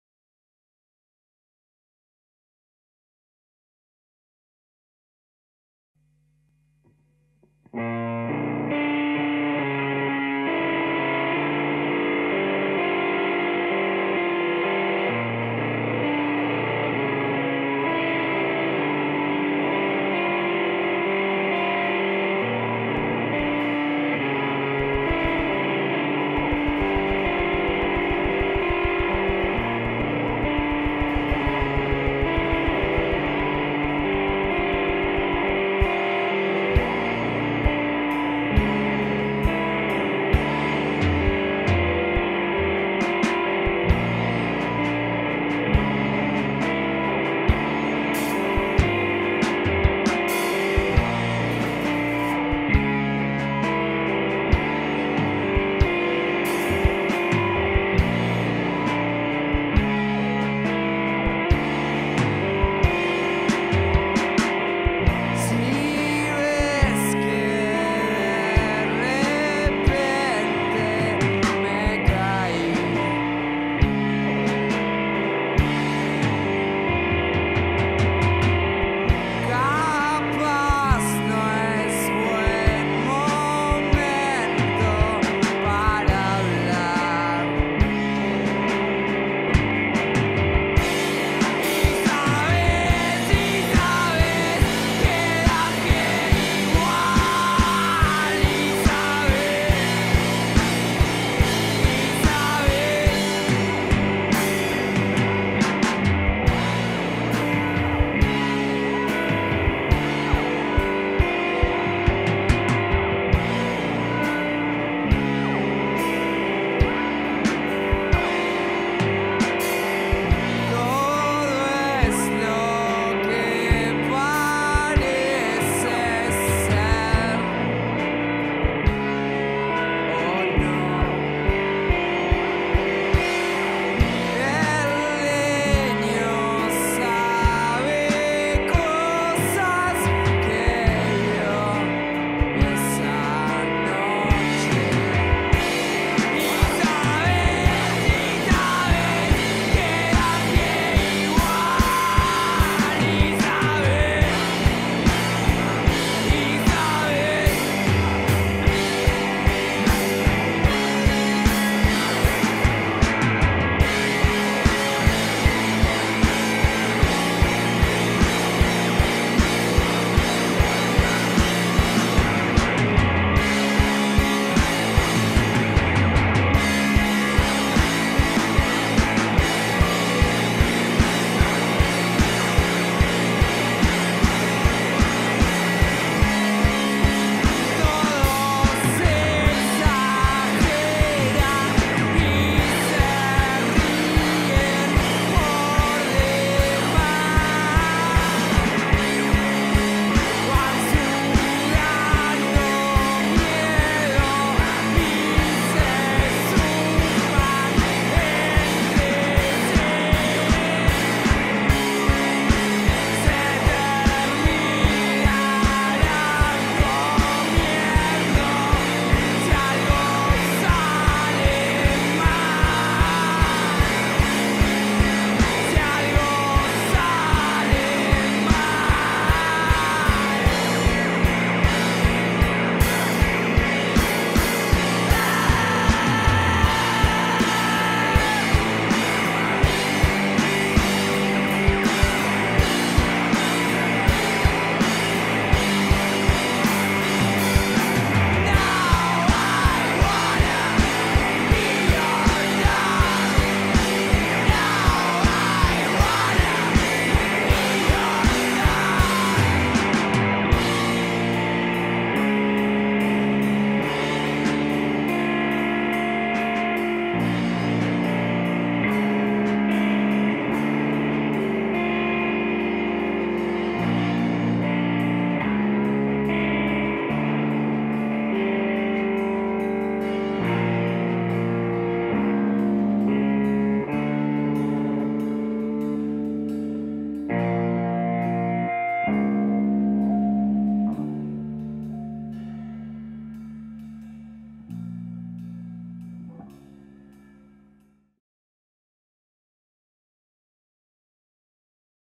Isabel - Power Chocolatín Experimento
Primer ciclo de entrevistas a bandas de 4 Cuarenta, realizada en sala Overdive

4Cuarenta
seguínos en las redes y no te pierdas de nada.

Power Chocolatin Experimento